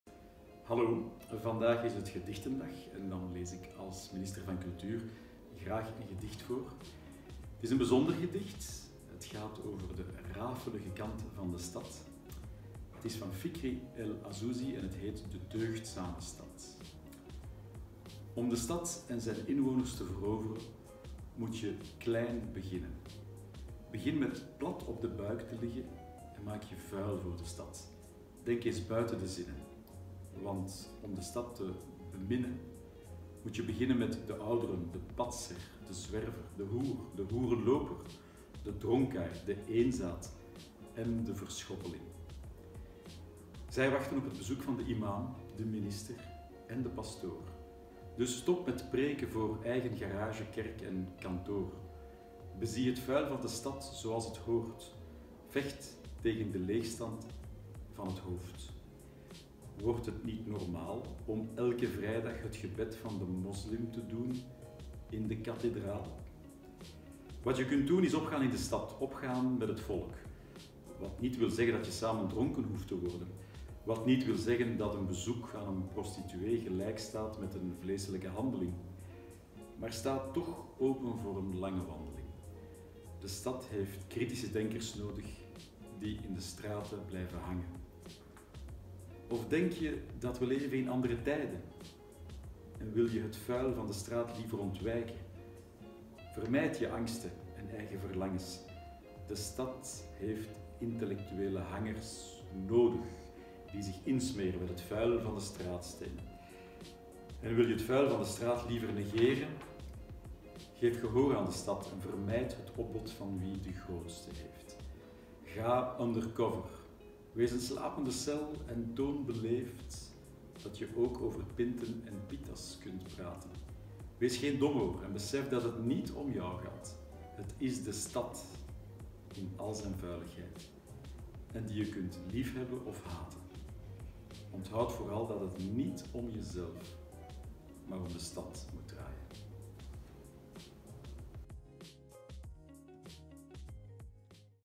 De deugdzame stad
Speciaal voor gedichtendag lees ik graag De deugdzame stad van Fikry El Azzouzi voor. Een gedicht over de rafelige kant van de stad.